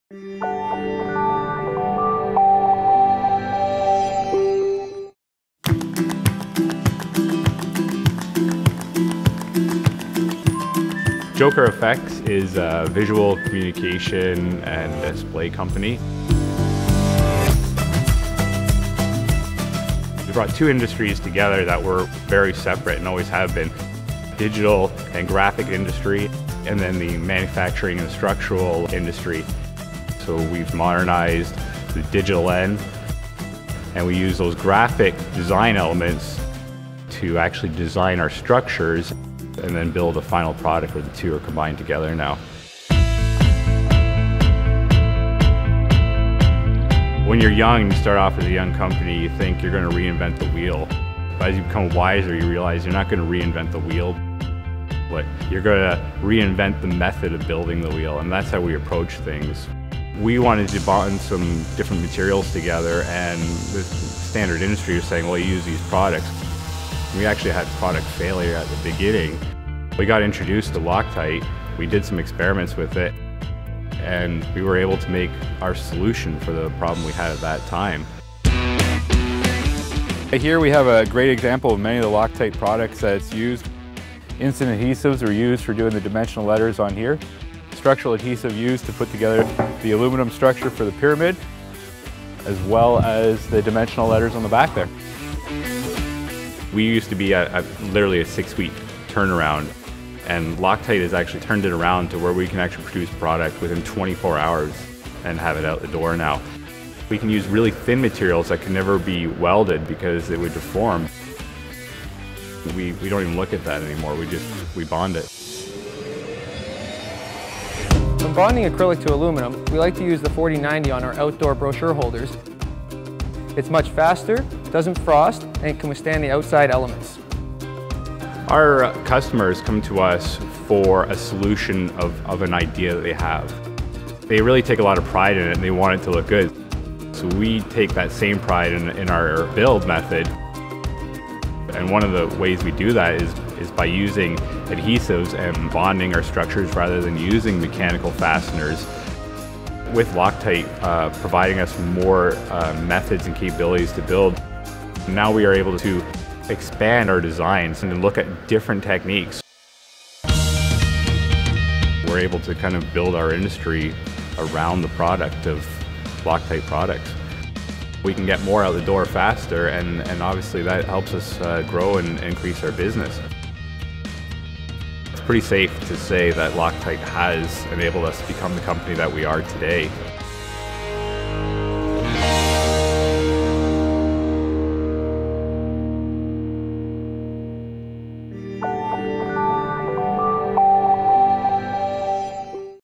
LOCTITE Success Story: Joker Fx
At Joker FX, a visual communications company, digital design meets structural manufacturing through the use of LOCTITE® Adhesives. The strength, reliability and durability of LOCTITE® adhesives, including LOCTITE 4090, have not only improved production times, but have helped Joker FX grow their business.

To watch how other companies are driving innovation with LOCTITE® adhesives or for product information,